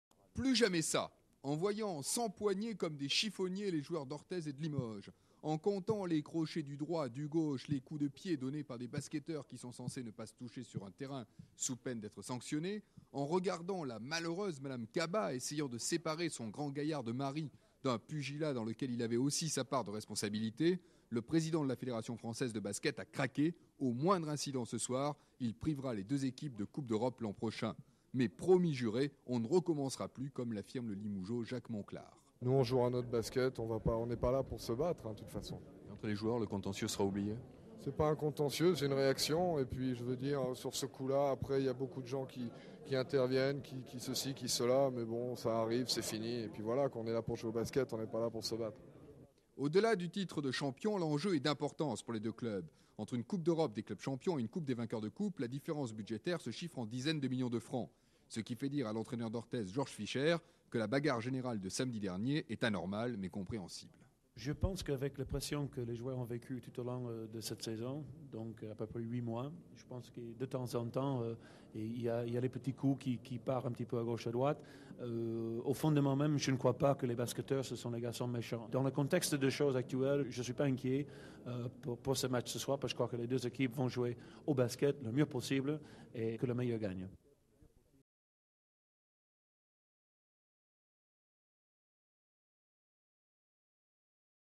Jacques Monclar : "on n'est pas là pour se battre"
ARCHIVES E1 - En 1987, le Limougeaud Jacques Monclar calme le jeu avant la belle de la finale du championnat entre le CSP et Orthez. Sujet de Jean-Yves Dhermain, diffusé à l'antenne d'Europe 1 le 5 mai 1987.